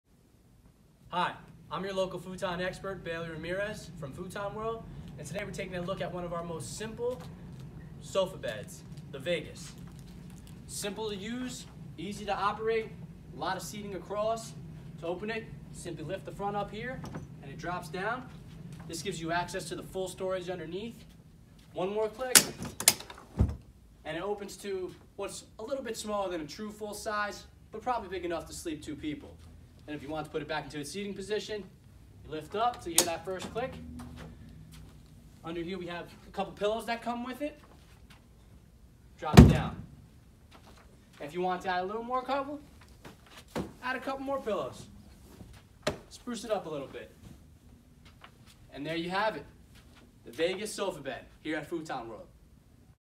The Vegas Sleeper Sofa by Istikal at Futon World NJ
The Vegas Sleeper Sofa by Istikbal at Futon World

Modern features and practical functionality makes the Vegas sofa one of our most popular models.  It’s made for sitting, sleeping and storing. It’s available in a wide variety of colors and includes two rectangular pillows that can double as armrests. It’s clean and simple styling fits neatly in any room. Easy opening click clack mechanism makes converting a breeze, easy enough if you’re 1 or 100.

Dimensions:

Sofa: 78.4″L x 35.4″D x 38″H

Bed Size: 78.4″L x 47.2″D

If you have kids, family, friends or overnight guests YOU need a futon.  Choose yours today